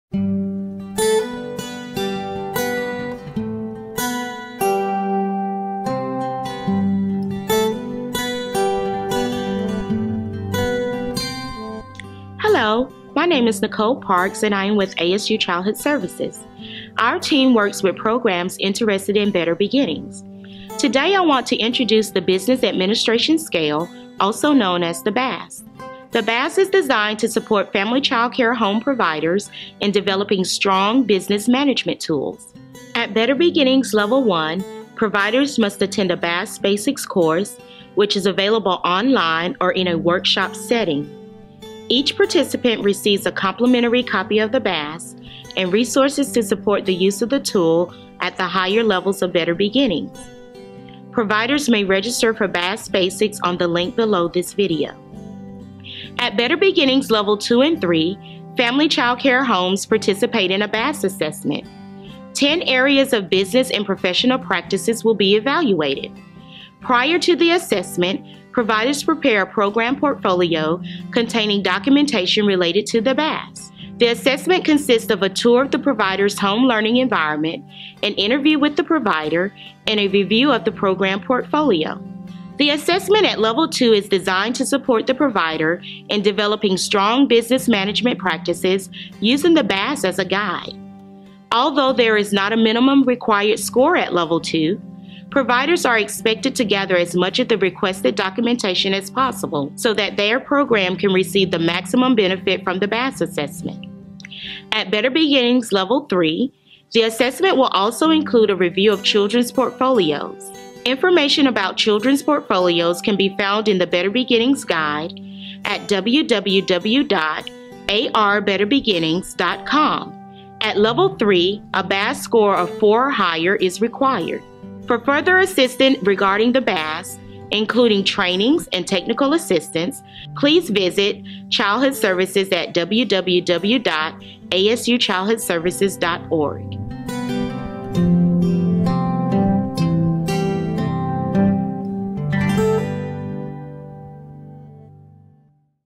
Business Administration Scale Overview | arbetterbeginnings.com
The Business Administration Scale, also known as the BAS, is designed to support family child care home providers in developing strong leadership and management practices. At Better Beginnings Level 1, the provider completes a BAS Basics course. Better Beginnings Levels 2 and 3 require family child care programs to participate in a BAS assessment.

Register for BAS Basics or other training Click on the “Training Opportunities” link in the menu and search for the appropriate training.

For more information about Better Beginnings, please visit our